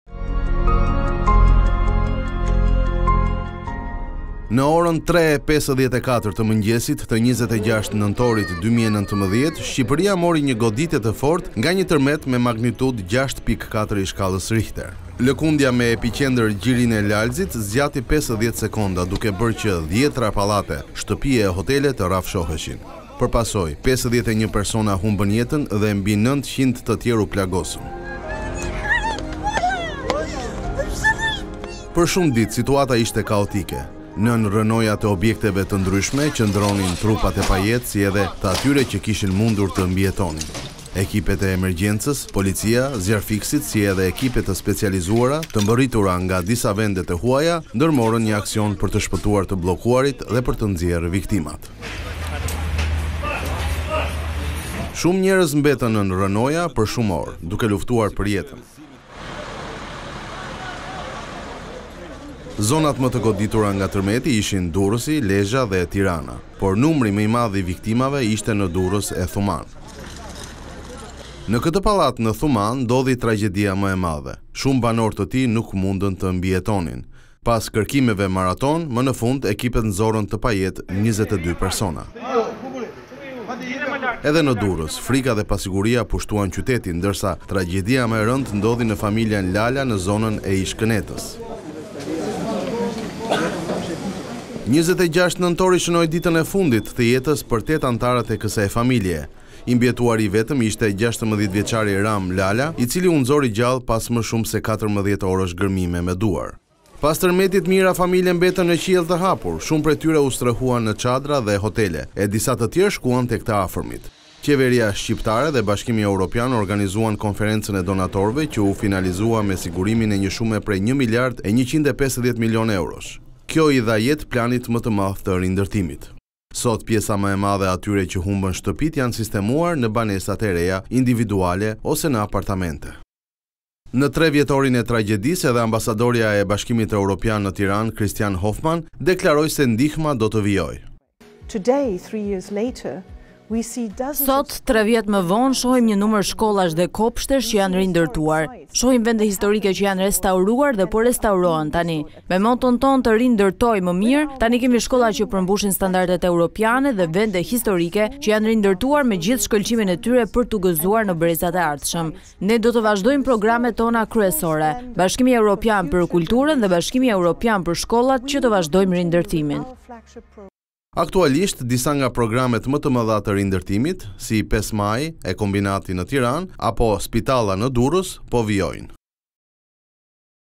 Tre vjet nga termeti tragjik. Lekundjet lane pas 51 te vdekur e qindra te plagosur -
Në orën 03:54 te mëngjesit të 26 nëntorit 2019, Shqipëria mori një goditje të fortë nga një tërmet me magnitudë 6.4 i shkallës rihter.Lëkundja me epiqendër gjirin e Lalzit zgjati 50 sekonda, duke bërë që dhjetëra pallate, shtëpi e hotele, të rrafshoheshin. Për pasojë, 51persona humbën jetën dhe mbi 900 të tjerë u plagosën.Për shumë ditë, situata ishte kaotike. Nën rrënojat e objekteve të ndryshme qëndronin trupat e pajetë si dhe të atyre që kishin mundur të mbijetonin. Ekipet e emergjencës, policia, zjarrfikësit si dhe ekipe te specializuara të mbërritura nga disa vende të huaja, ndërmorën një aksion për të shpëtuar të bllokuarit e për të nxjerrë viktimat. Shumë njerëz mbetën nën rrënoja për shumë orë, duke luftuar për jetën Zonat më të goditura nga tërmeti ishin Durrësi, Lezha dhe Tirana. Por numri më i madh i viktimave ishte në Durrës e Thumanë.Në këtë pallat në Thumanë ndodhi tragjedia më e madhe. Shumë banorë të tij nuk mundën të mbijetonin. Pas kërkimeve maratonë, më në fund ekipet nxorën të pajetë 22 persona. Edhe në Durrës , frika dhe pasiguria pushtuan qytetin, ndërsa tragjedia më e rëndë ndodhi në familjen lala në zonën e ish-kënetës. 26 nëntori shënoi ditën e fundit të jetës për tetë anëtarët e kësaj familjeje. I mbijetuari i vetëm ishte 16 vjeçari Ramë Lala, i cili u nxor i gjallë pas më shumë se 14 orësh gërmime me duar.Pas tërmetit, mijëra familje mbetën në qiell të hapur: shumë prej tyre u strehuan në çadra dhe hotele, e disa të tjerë shkuan tek të afërmit. Qeveria shqiptare dhe Bashkimi Europian organizuan konferencën e Donatorëve që u finalizua me sigurimin e një shume prej 1 miliardë e 150 milionë eurosh. Kjo i dha jetë planit më të madh të rindërtimit. Sot, pjesa më e madhe e atyre që humbën shtëpitë, janë sistemuar në banesat e reja individuale ose në apartamente. Në tre vjetorin e tragjedisë, edhe ambasadorja e Bashkimit Europian në Tiranë, Kristiane Hohman deklaroi se ndihma do të vijojë.Sot, tre vjet mw vonw shohim njw numër shkollash dhe kopshtesh qw janw rindërtuar, shohim vende historike qw janw restauruar dhe po restaurohen tani. Me moton tonw ‘Tw rindwrtojmw mw mirw’ tani kemi shkolla qw përmbushin standardet evropiane dhe vende historike qw janw rindërtuar me gjithw shkëlqimin e tyre pwr t’u gwzuar nw brezat e ardhshwm. Ne do tw vazhdojmë programet tona kryesore: ‘BE pwr kulturwn’ dhe ‘BE pwr shkollat’ qw tw vazhdojmë rindërtimin. Aktualisht, disa nga programet më te mëdha të rindërtimit si 5 maji e kombinati në Tiranë apo Spitalla në Durrës, po vijojnë.